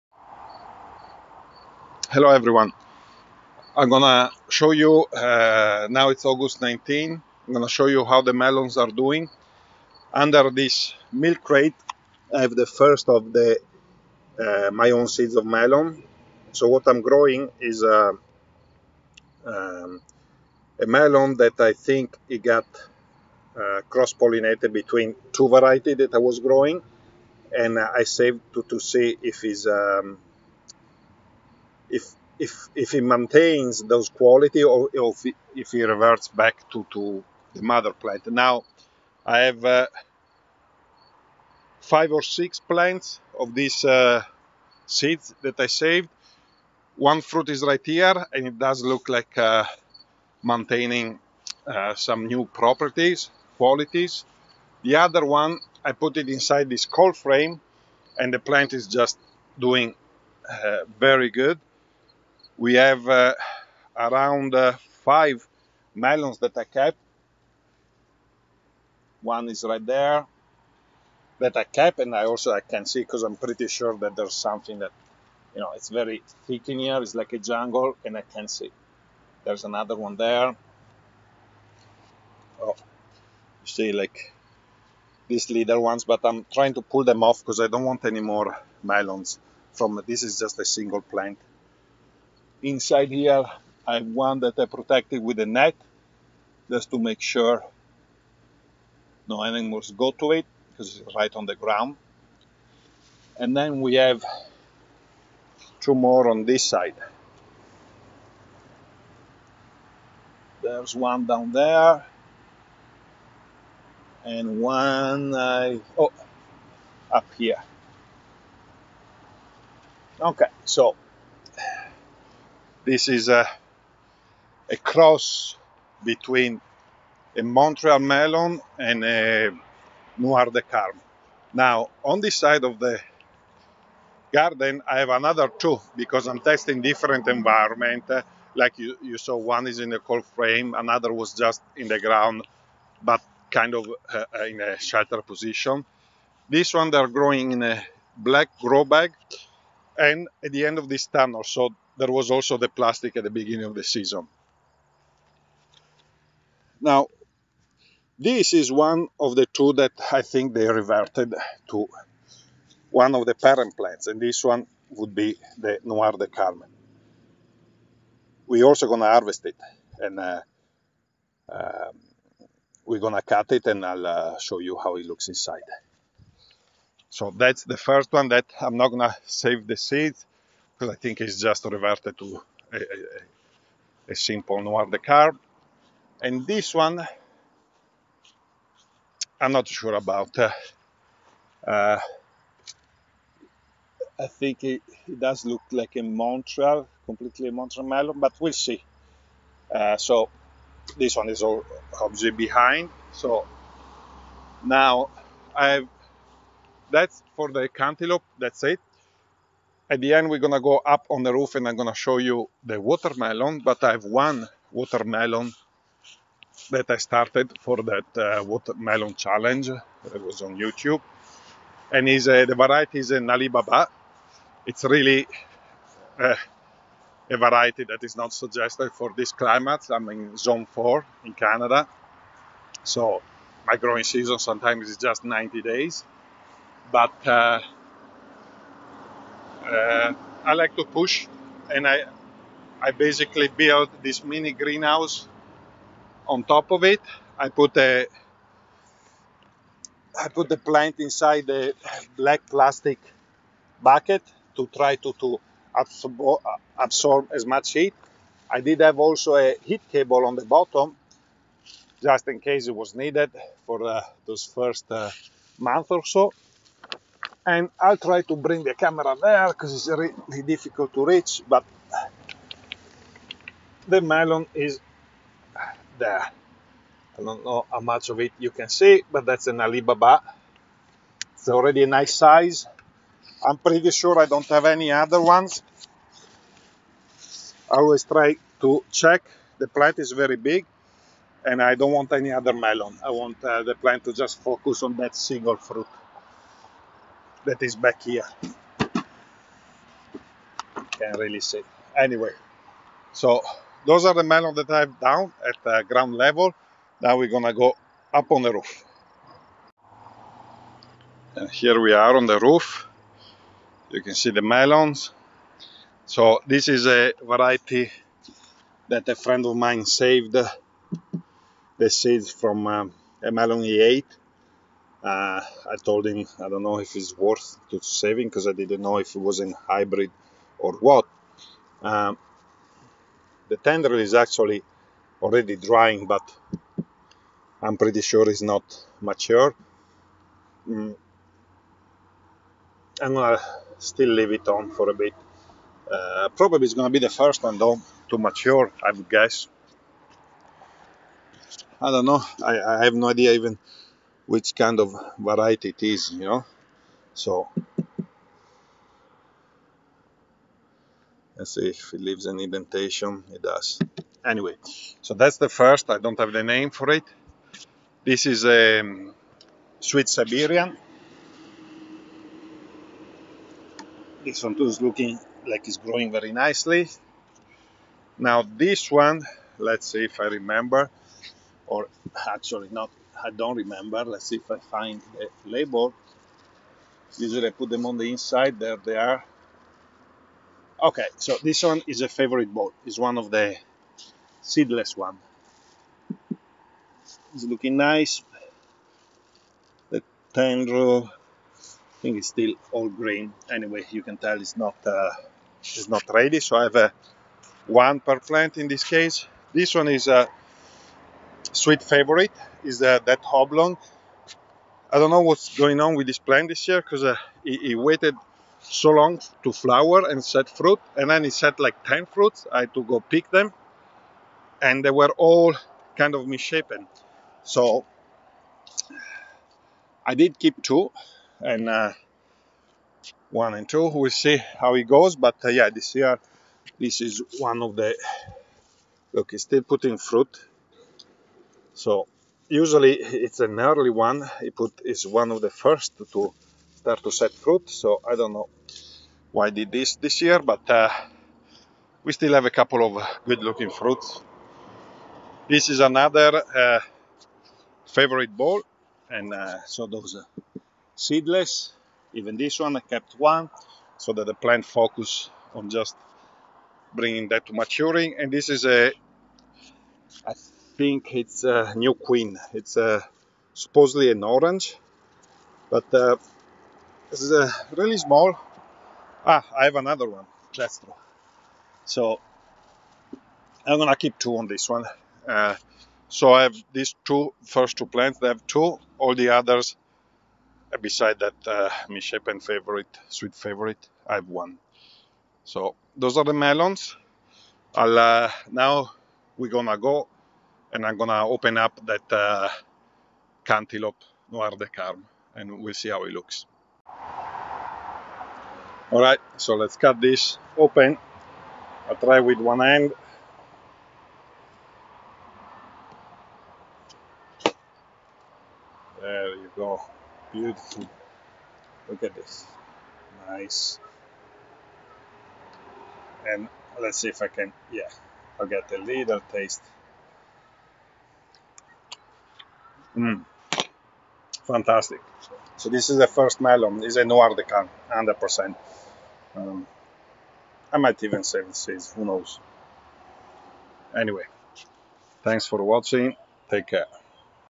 Growing melons in Canada
Melon season 2021
This year I'm growing different varieties of watermelon, but only one of cantaloupe ( my own seeds )
I'm in zone 4, Canada